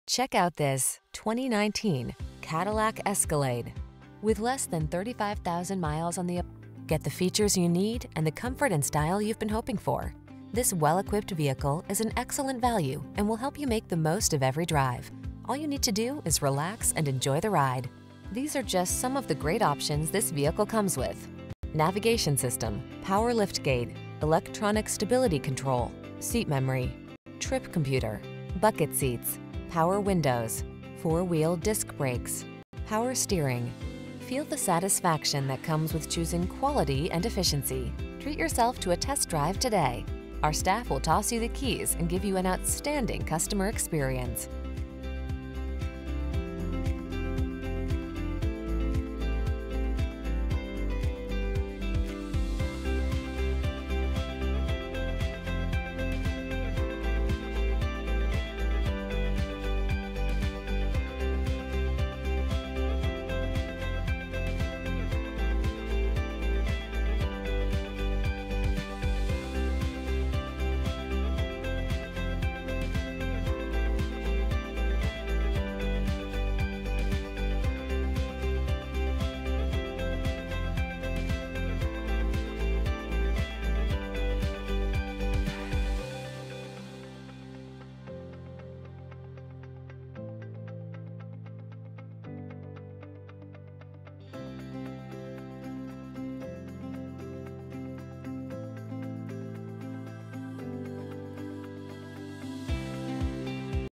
2019 Cadillac Escalade Miami Shores, Aventura, Miami Beach, Miami Lakes, Miami, FL P3082F
Satin Steel Metallic Used 2019 Cadillac Escalade available in Miami Shores, Florida at Tropical Chevrolet. Servicing the Aventura, Miami Beach, Miami Lakes, Miami, FL area.

Tropical Chevrolet
8880 Biscayne Blvd

2019 Cadillac Escalade Base Recent Arrival! Odometer is 2653 miles below market average!brAwards:br  * JD Power Initial Quality Study